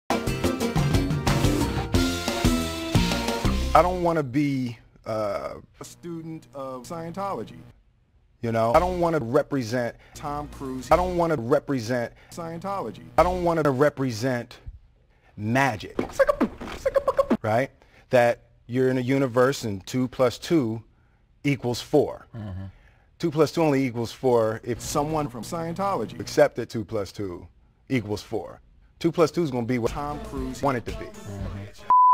Televised Confession of Will Smith!
here is part two of newly discovered cutting room floor footage of Will Smith denouncing the madness and quackery that is scientology!

he just may save his career!